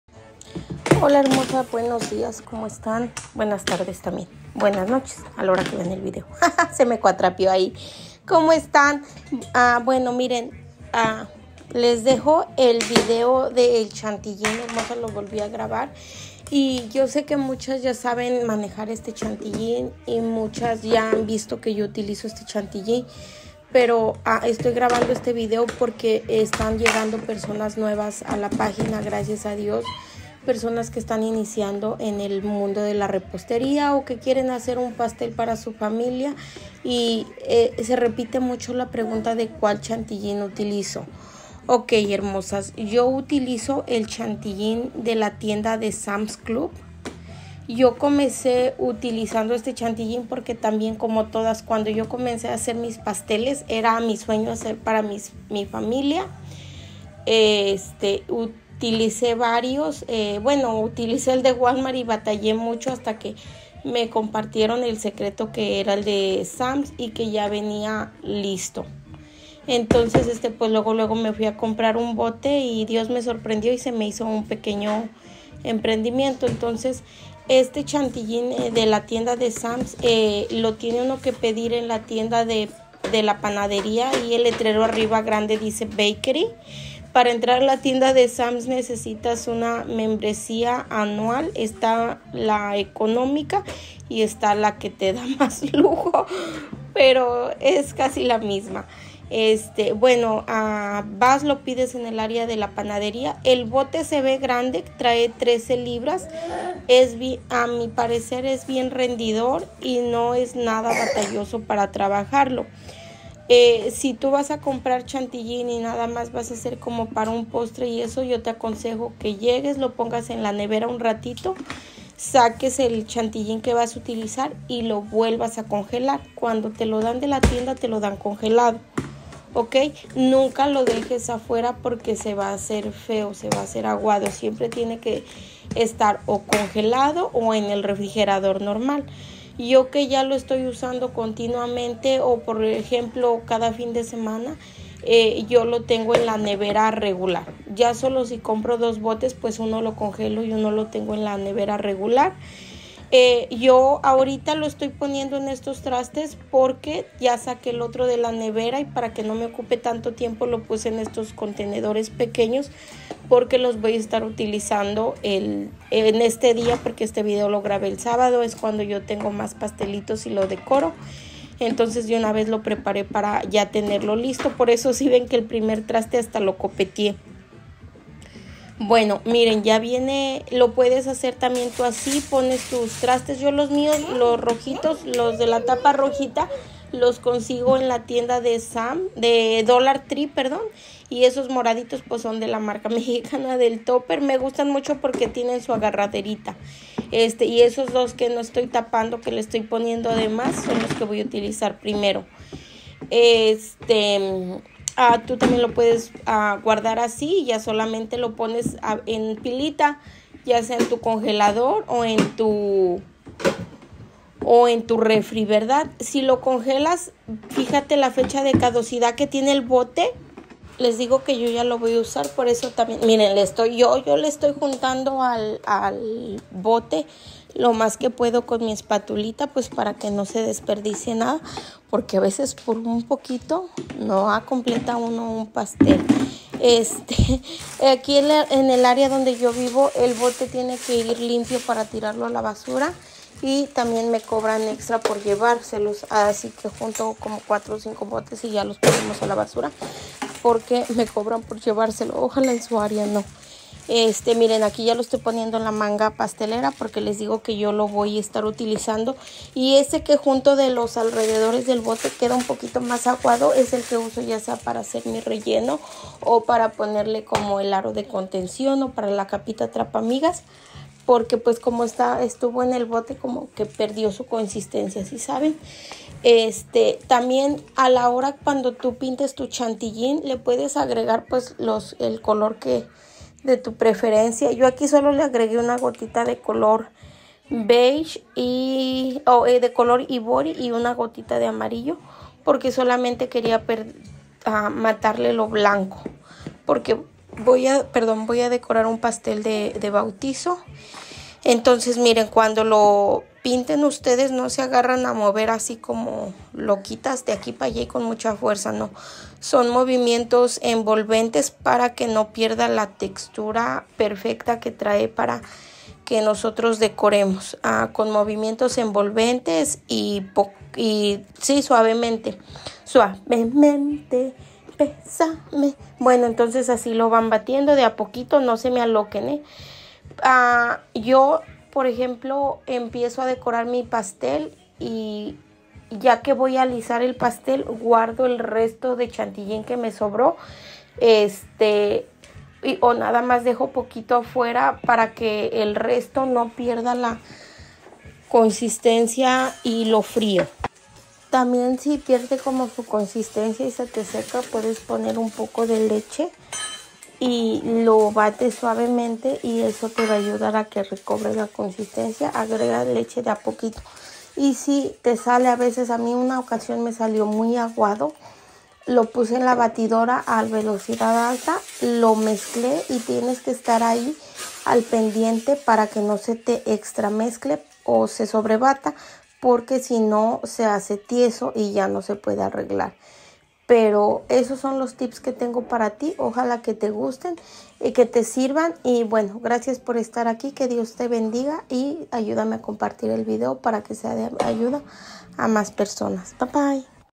Chantilly de Sam’s
Q Hola hermosa les dejo este video de el Chantilly que yo utilizó un poquito más explicado si tienen alguna duda déjeme los comentarios y por favor ayúdenme a compartir